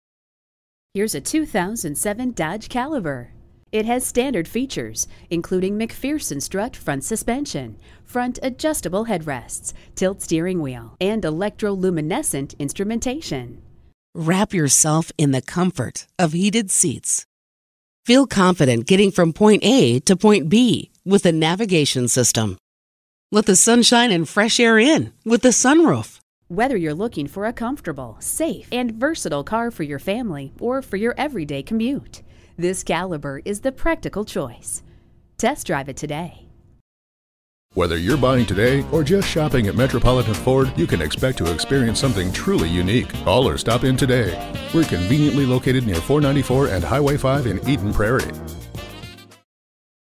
2007 Dodge Caliber Minneapolis MN Eden Prairie, MN 160102B12 - SOLD
Year: 2007
Make: Dodge
Model: Caliber
Trim: 4dr HB SXT FWD 2.0L 4Cyl Nav
Engine: 2.0 L 4 Cyl. Cyl.
Transmission: Automatic
Color: Inferno Red Crystal Pearl
Interior: Pastel Slate Gray
Mileage: 103016
Stock #: 160102B12
VIN: 1B3HB48B37D259399

Address:
12477 Plaza Drive